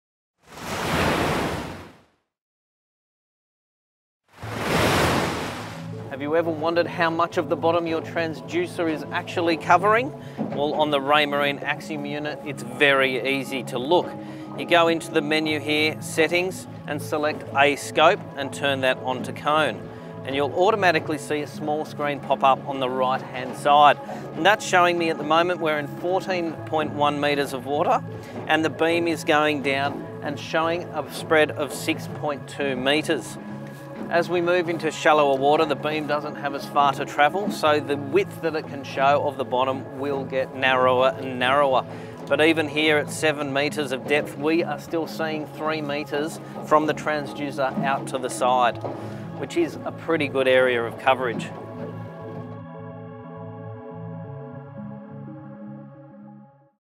Raymarine Axiom How To: Using A-scope to Visualize Width of Your Cone
Explaining how using A-Scope on your Raymarine Axiom can show the area the sonar channel is covering.